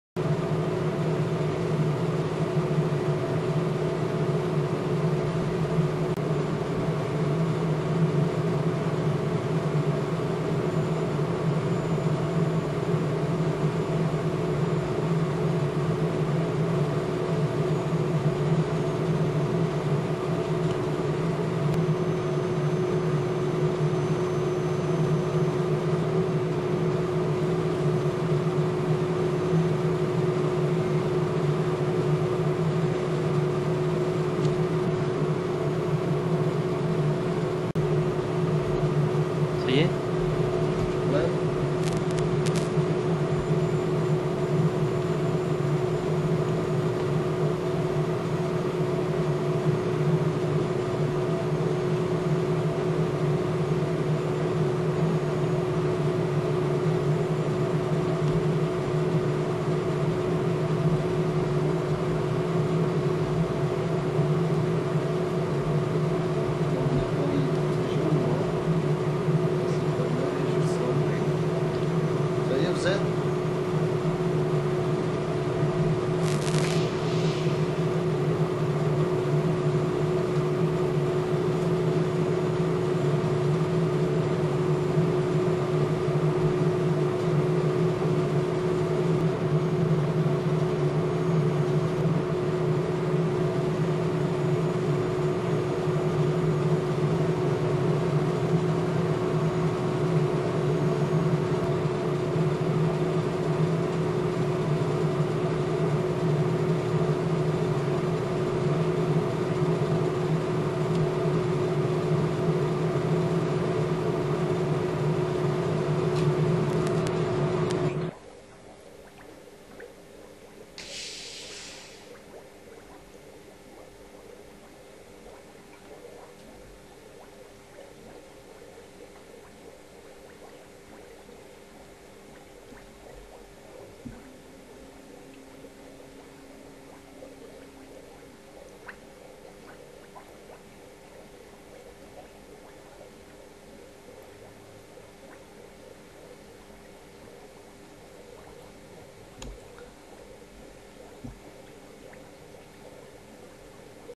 Rheocooling Solidifying 1 liter of water in less than 5 seconds
I want to demonstrate Rheo-cooling with an example: the solidification of one liter of water contained in a round flask in about 5 seconds! I actually made the experiment and it worked. The video below is amazing to watch and illustrate the idea.